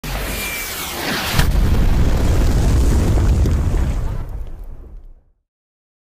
CEDRIC GETS BLOWN UP
RUNNNNN! As Jamal and Jake run cedric is left clueless and a missile blows him up. Then Chris randomly comes in confused xD